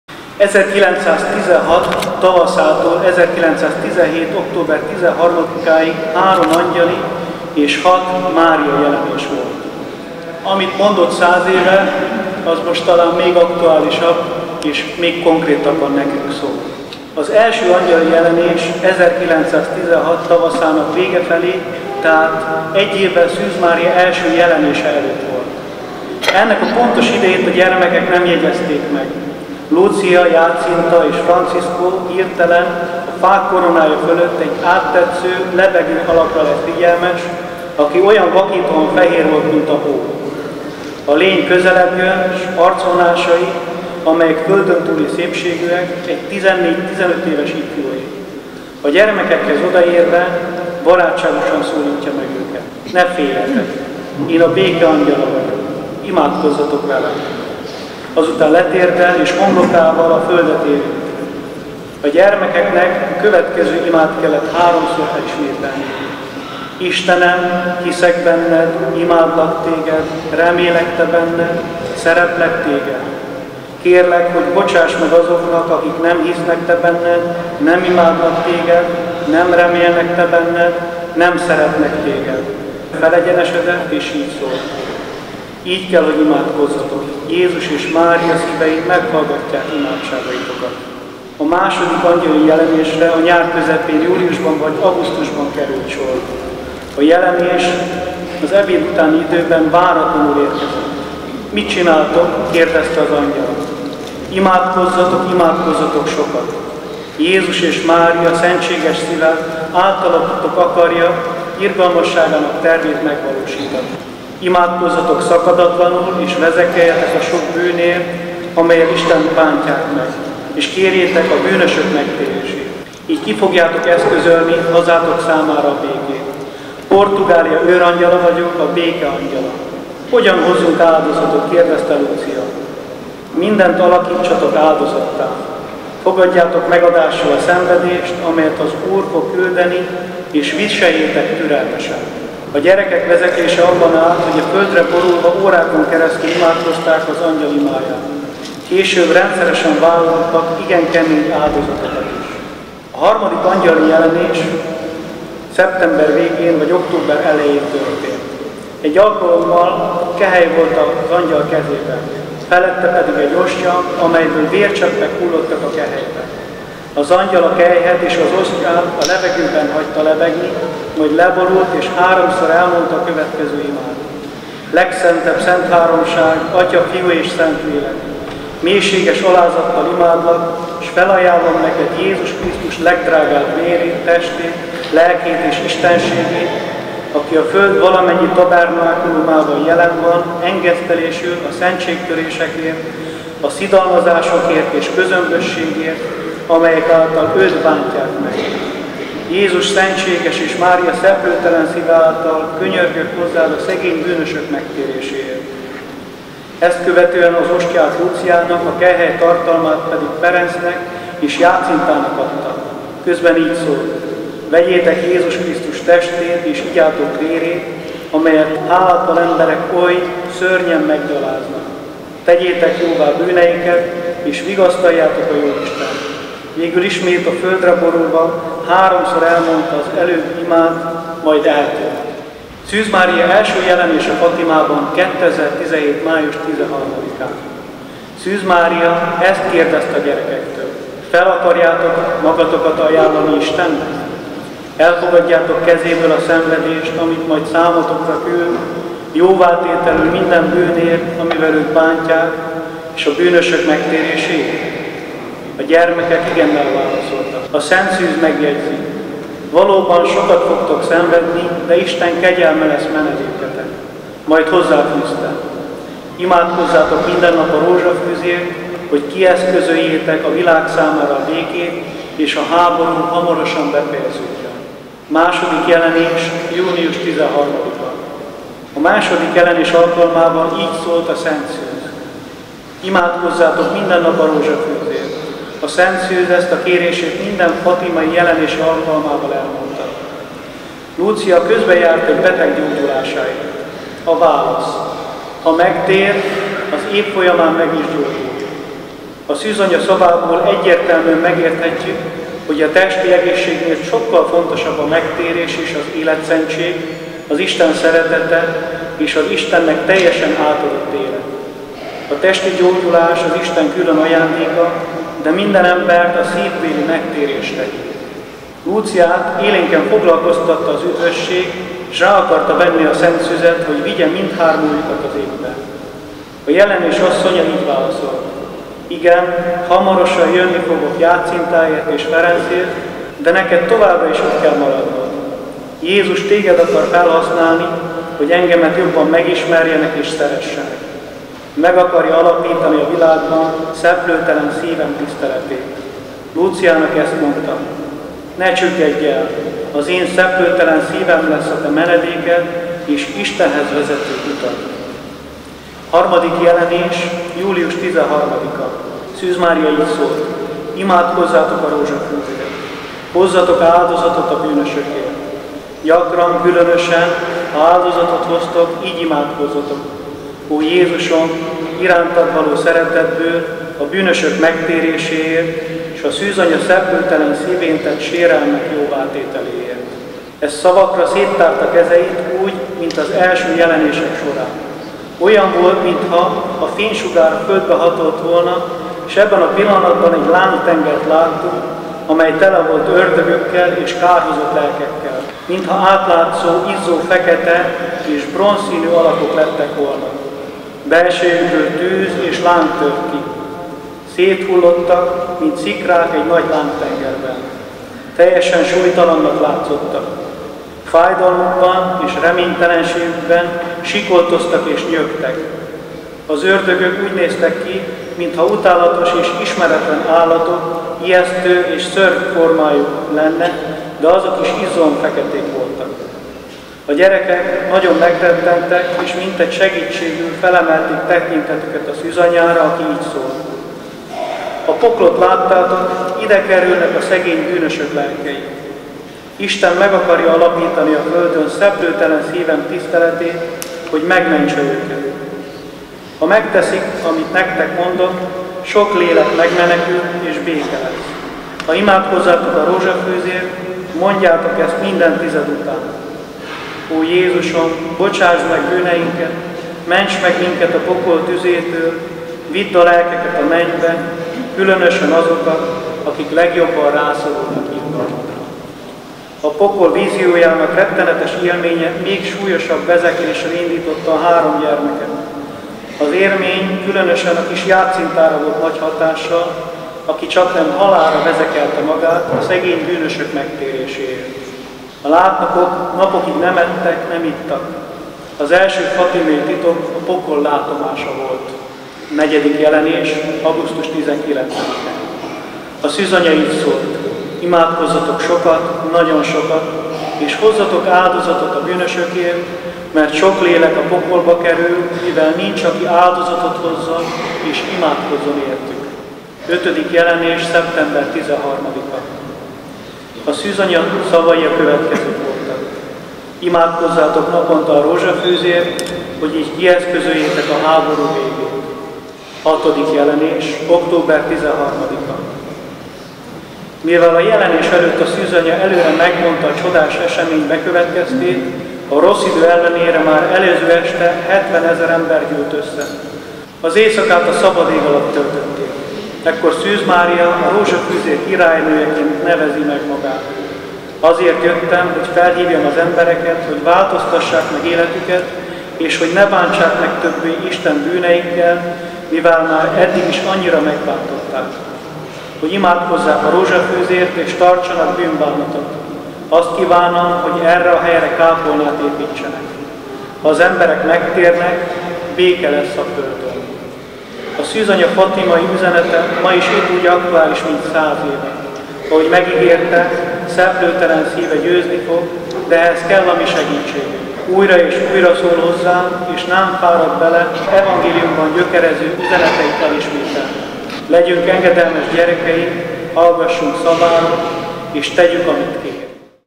A fatimai jelenések üzenete
A fatimai jelenéseket, s azok üzeneteit vette sorra Maksó Péter diakónus az egri Rózsafüzér Társulatok Találkozóján. Hallhatjuk az angyal s a Szűzanya szavait; ,,nézhetjük" a pokoli látomást, amely még súlyosabb vezeklésre indította a három gyermeket a szegény bűnösök megtéréséért; s lélekben ott lehetünk a kisvideó közreadásának napjától (október 13.) pontosan 100 évvel korábbi Napcsodánál.
Ha tetszett a videónk, kérjük, ossza meg barátaival, ismerőseivel
További videóinkat keresse honlapunkon is: